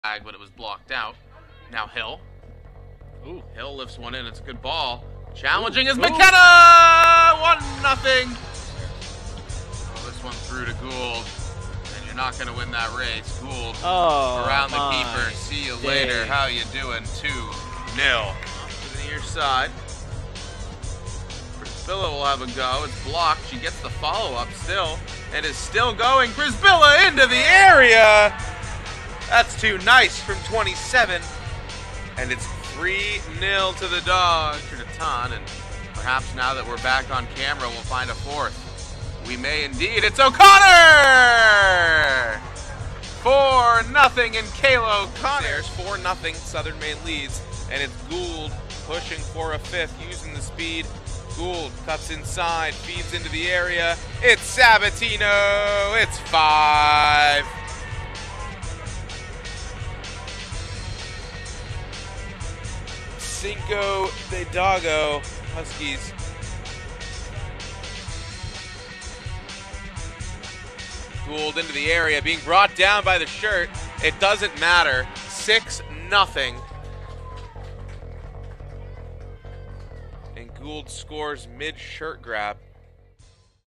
Husky Highlights: Women's Soccer 6, UMass Dartmouth 0
Huskies' women's soccer blanks UMD 6-0. Julia McKenna with the first goal/game-winner, followed by goals from Catriona Gould (2), Ashley Sabatino, Kayla O'Connor and Meghan Przybilla. Sebastian Pettit and Connor Blake on the call. Cover photo by Jesse Kamalandua, Athletic Comms Student Employee